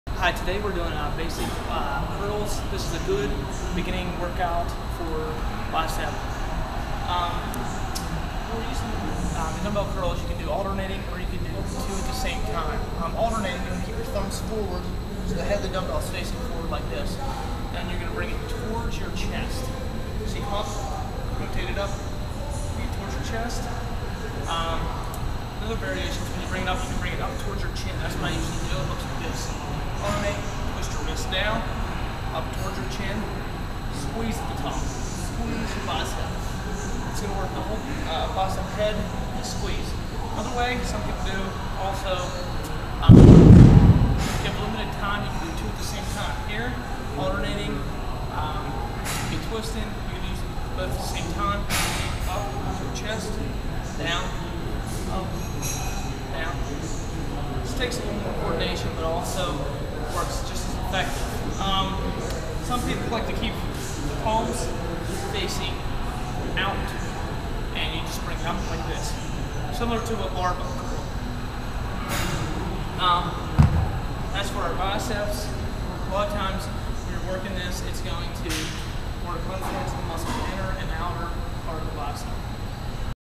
Zi6_1137.MOV
alternating dumbbell curl- intro. to weight lifting breaking down each step of the exercise.